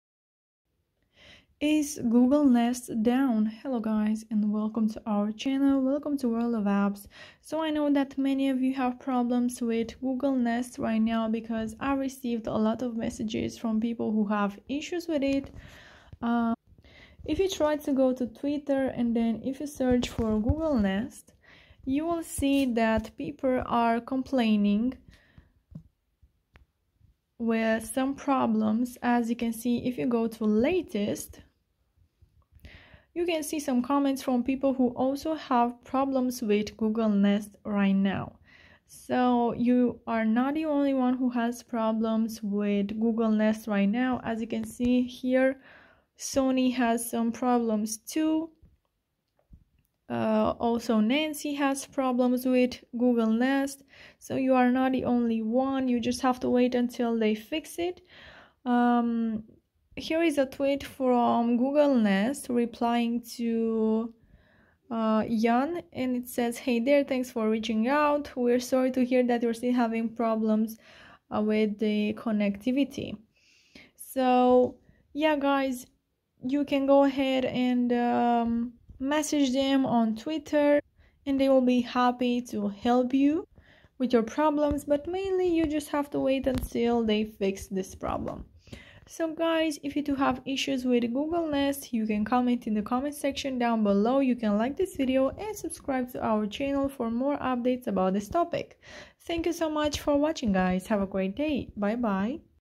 Google Nest is Down | Unable to login Error "There was a problem" How to fix Nest Servers
Google Nest is Down | Google Nest Connectivity issues, How to fix Google nest servers Down

Working 100%! How to fix Google Nest is Down | Google Nest Connectivity issues, How to fix Google nest servers Down